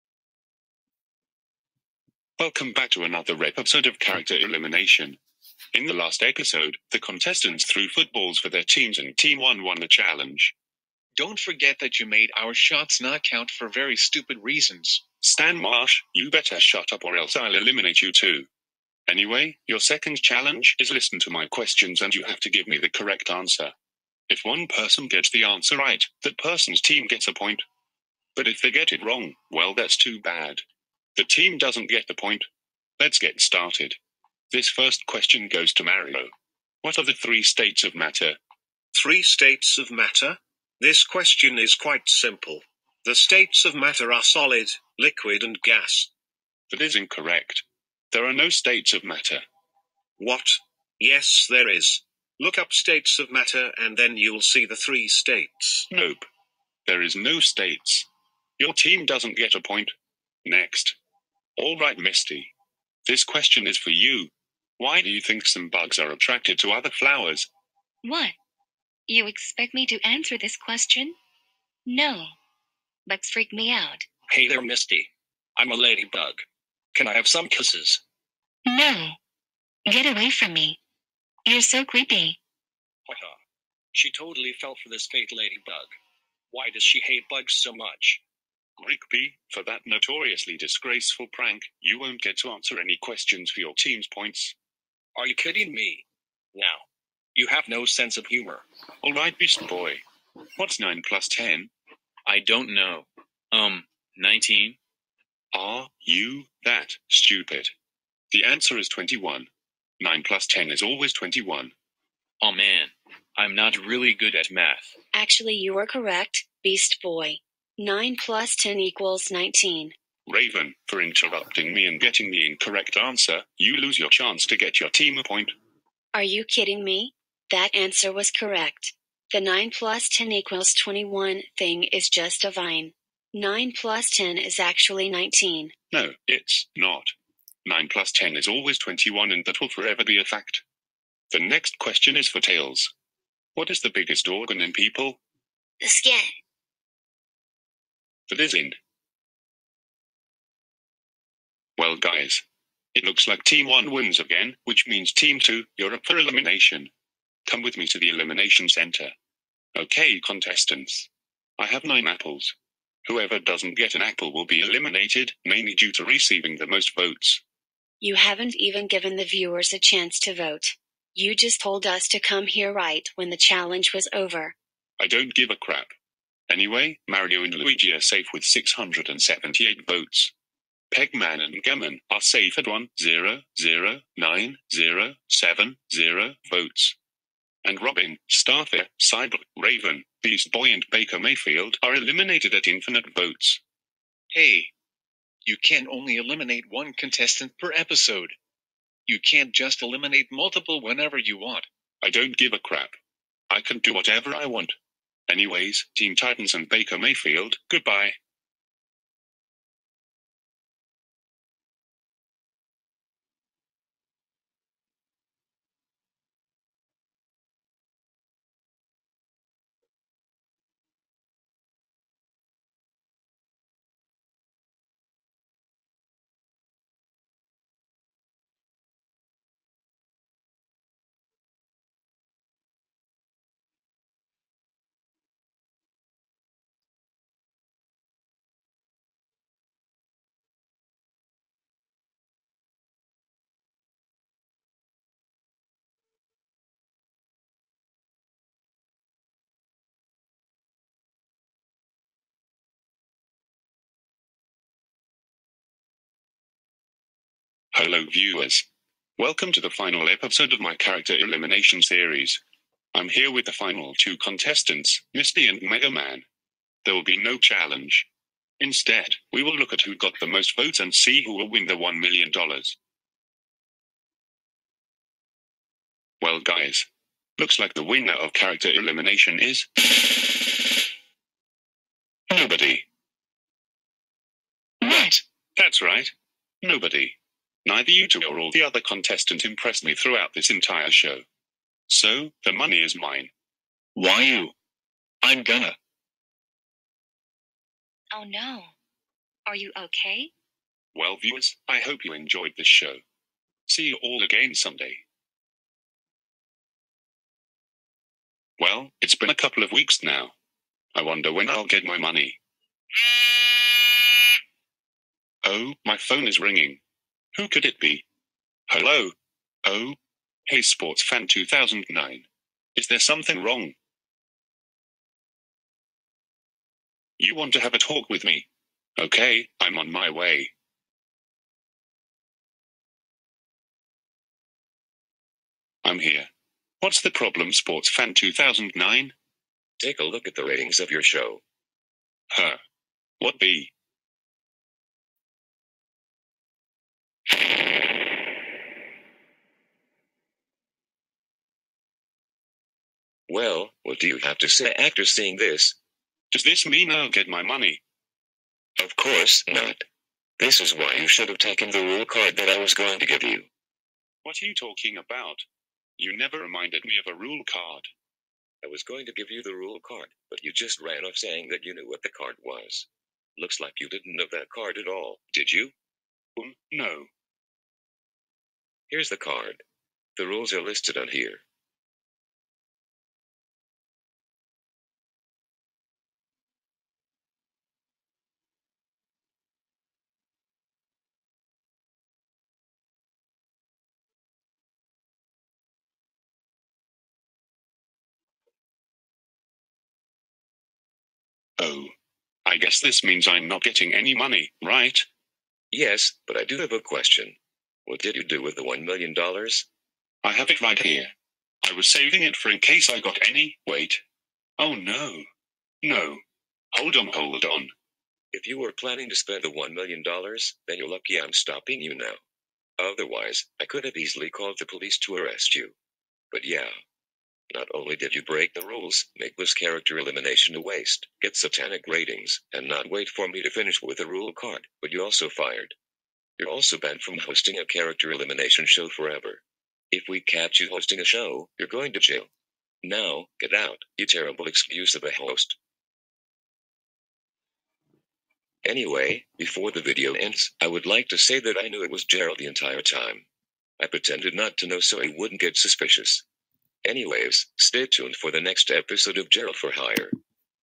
Gerald For Hire Episode 52: Character Elimination (Part 3)
This is the final part of the episode. Enjoy!
Note: I accidentally misspelled Elimination in the rules part. I’ll pay attention next time.